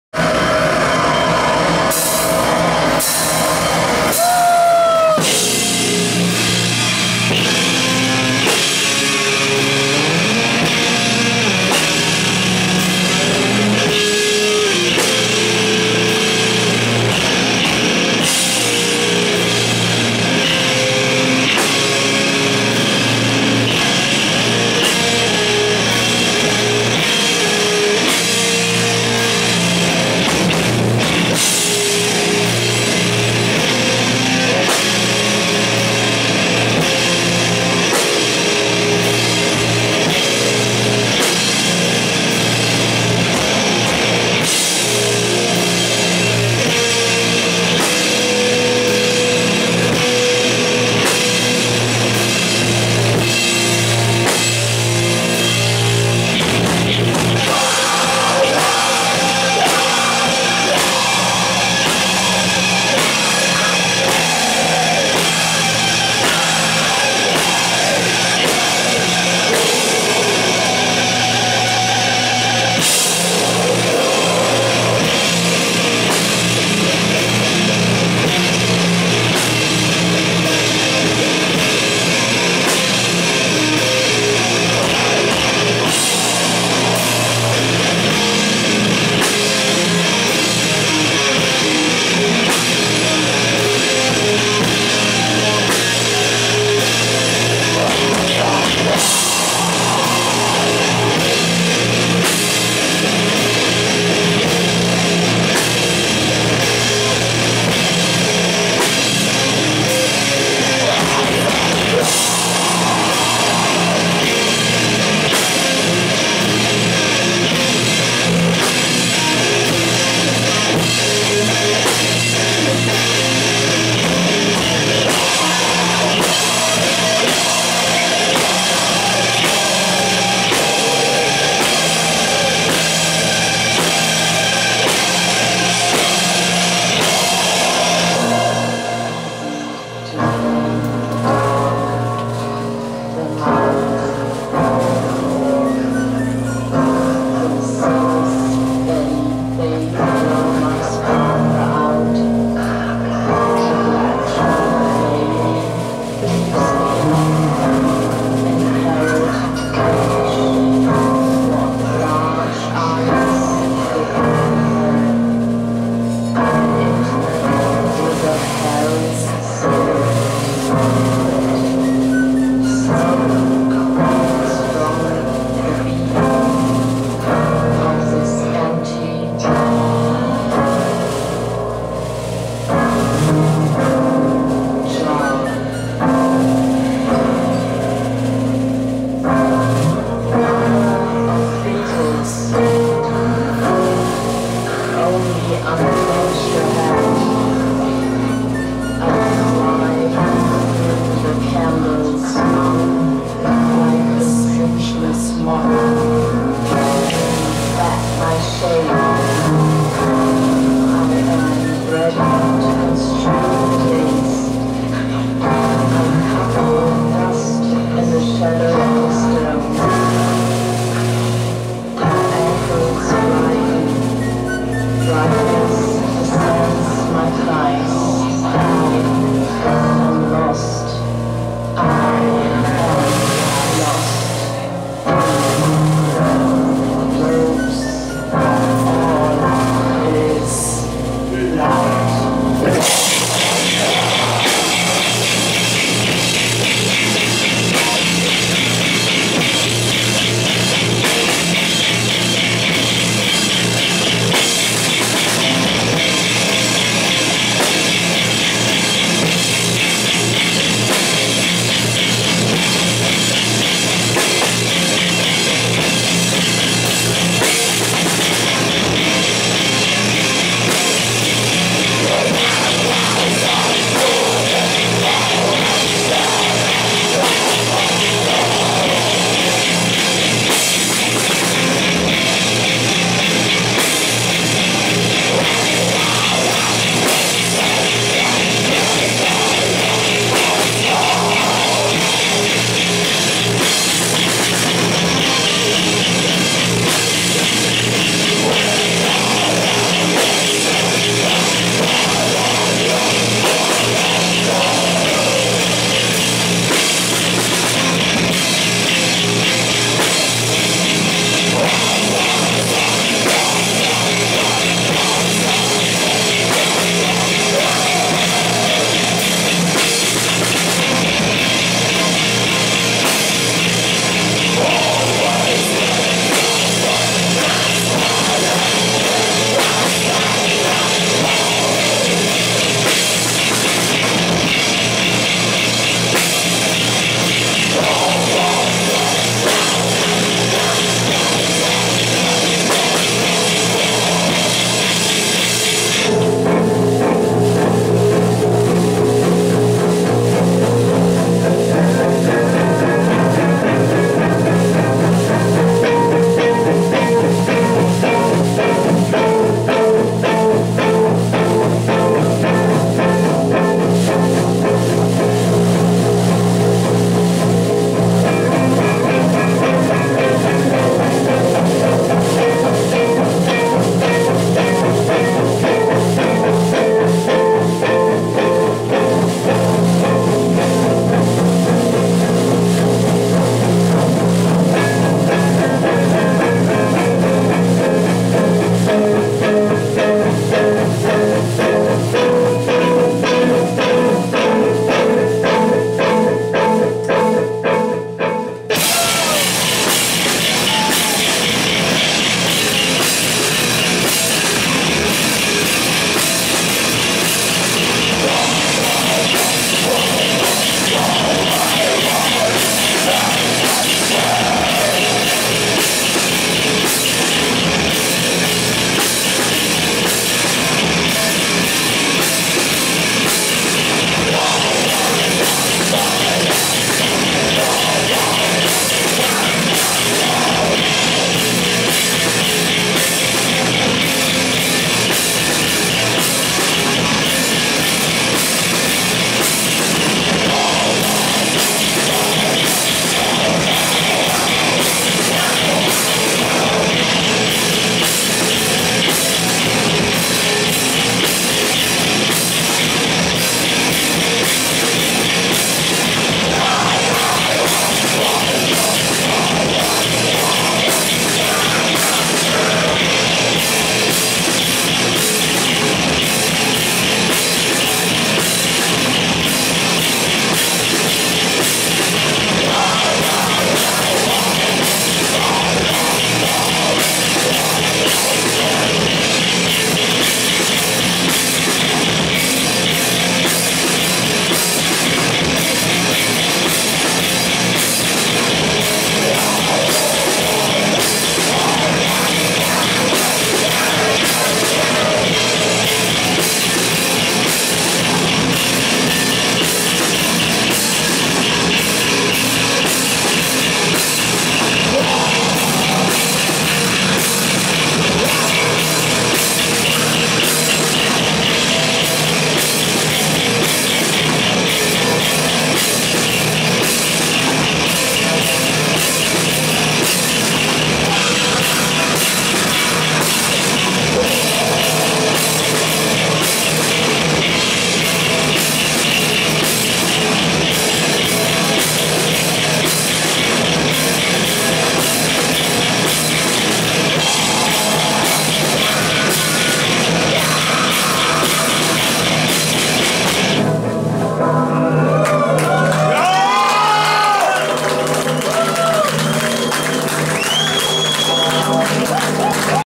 Phantom Winter @ Roadburn 2018 (1)
Phantom Winter playing live @ Roadburn in Tilburg, Netherlands (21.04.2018)